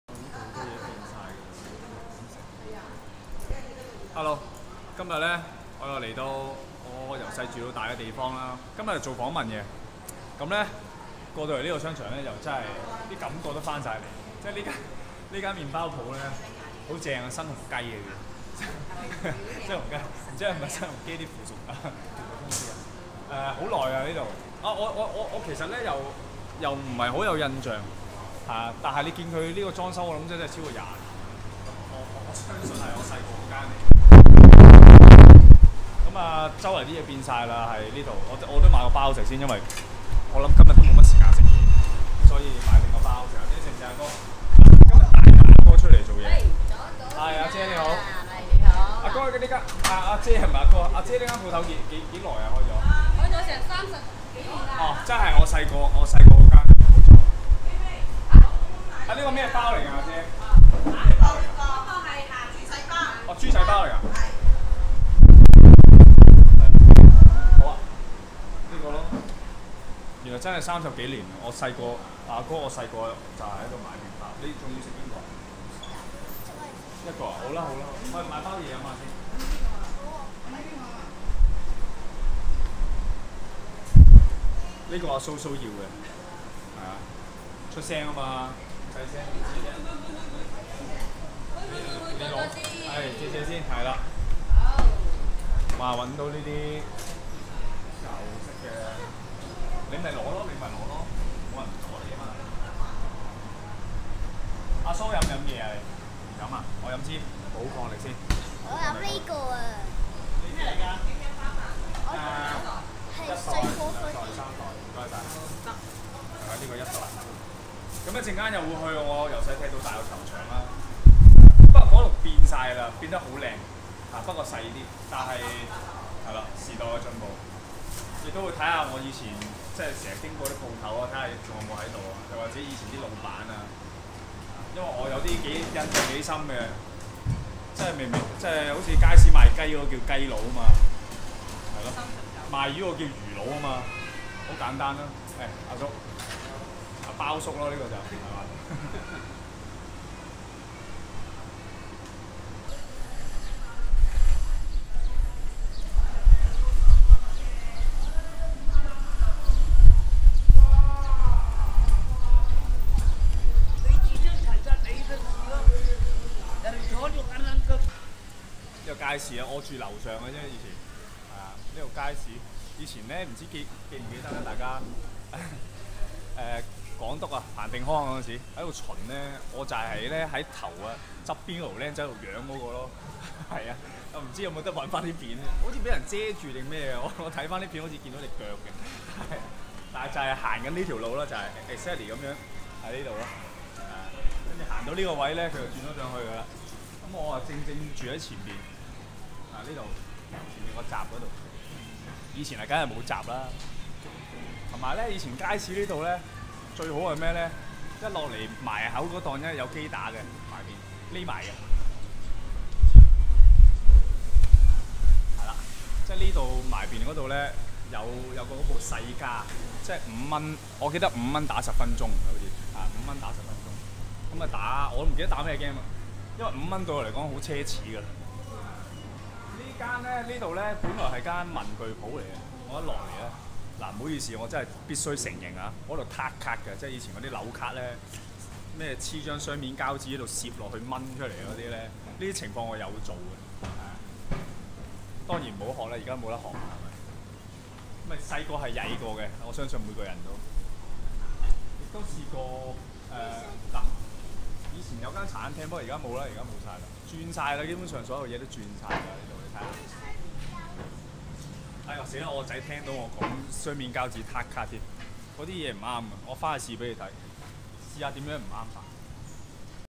【陳偉豪。講講講。我的童年生活#1】
第一集, 帶大家參觀吓樂華邨開左三十幾年嘅麵包舖同埋當年彭定康都來visit過嘅街市.